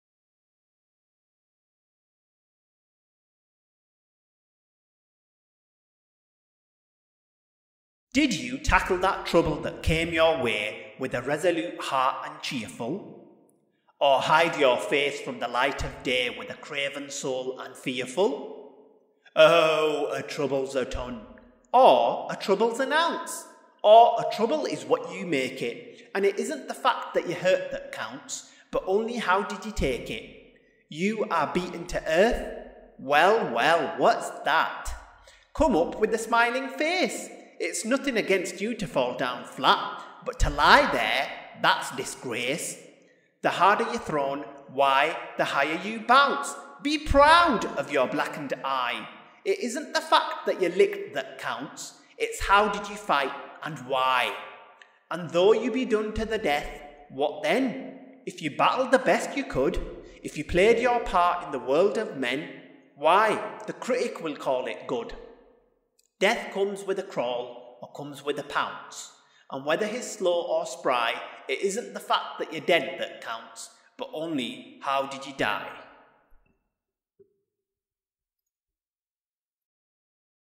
HOW DID YOU DIE? EDMUND VANCE COOKE - A MOTIVATIONAL VIDEO POEM FOR FIGHTING
Edmund Vance Cooke (June 5, 1866 – December 18, 1932) was a 19th- and 20th-century poet best remembered for his inspirational verse "How Did You Die?"

Cooke was born in Port Dover, Ontario. in 1898 he married Lilith Castleberry with whom he had five children. He later read his poems on radio, WWJ in Detroit, Michigan. He died in Cleveland, Ohio.

We hope you like this raw rendition with no background music. :-)